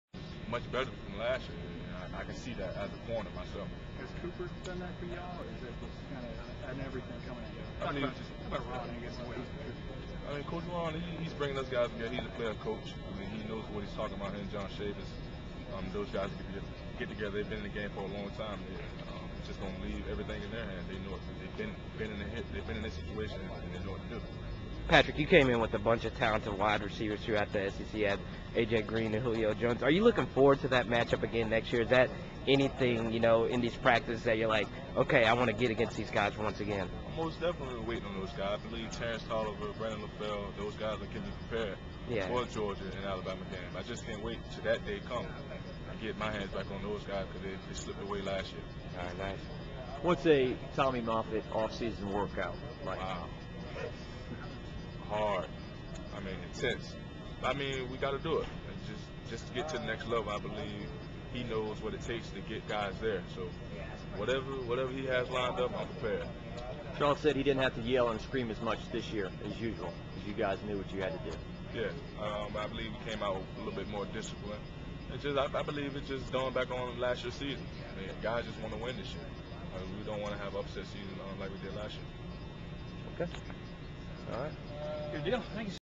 Patrick Peterson - LSU Practice 8-12-09
LSU CB Patrick Peterson talks about how LSU WR's Brandon Lafell and Terrance Tolliver gets him ready to face the top WR's in the conference.